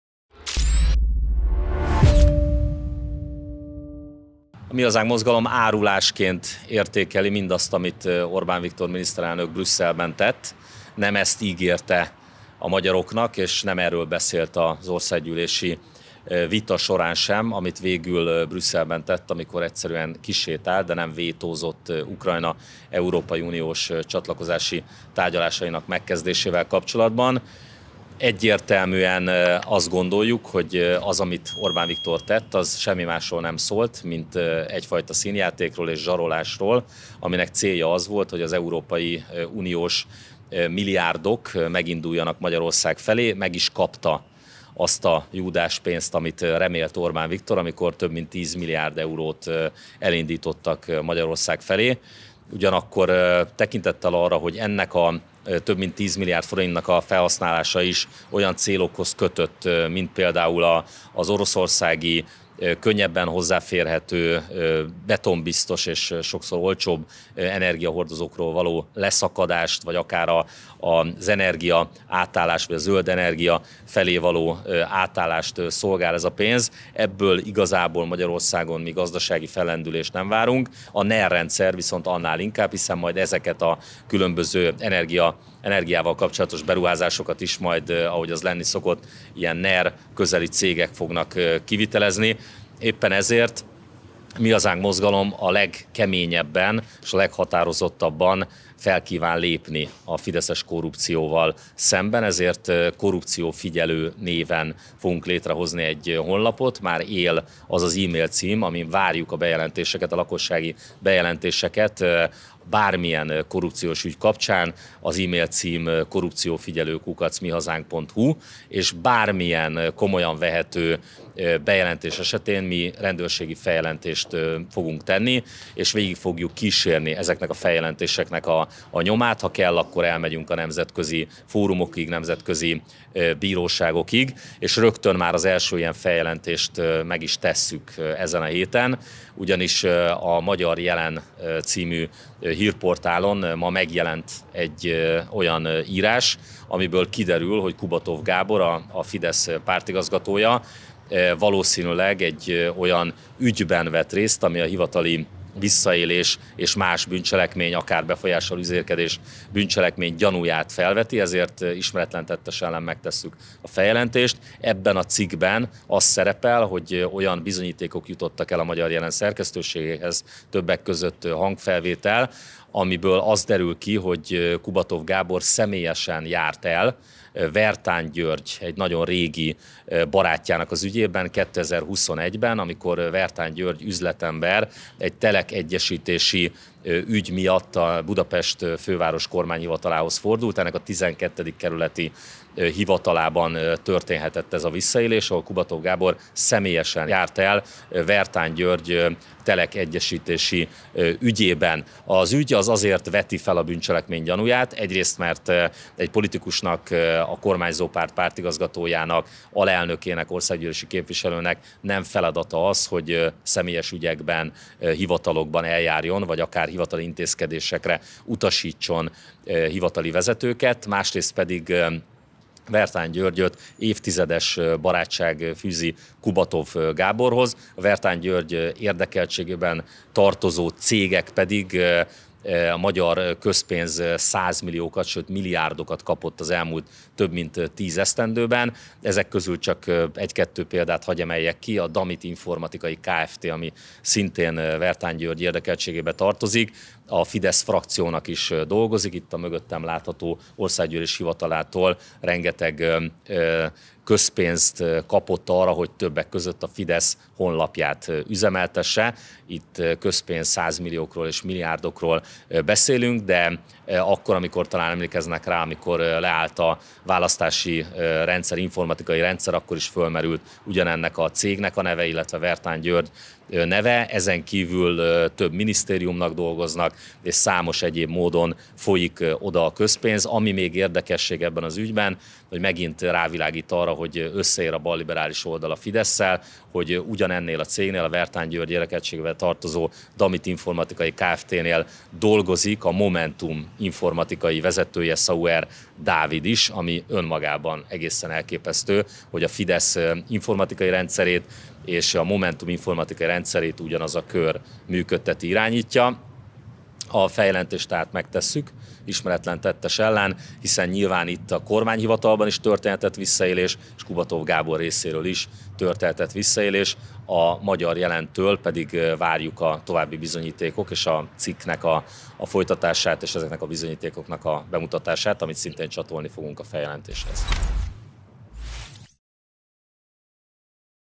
Korrupciófigyelőt állít fel a Mi Hazánk a most érkező uniós eurómilliárdok felhasználása kapcsán
Korrupciófigyelőt állít fel a Mi Hazánk a most érkező uniós eurómilliárdok felhasználása kapcsán. A sajtótájékoztatót tartja Toroczkai László, a Mi Hazánk elnöke.